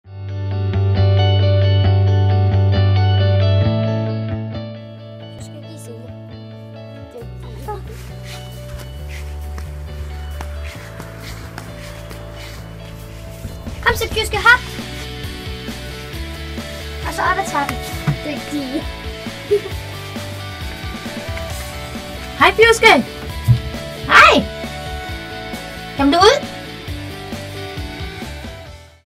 Et par tricks til Kanin
Min datter lærer sin kanin et par tricks:)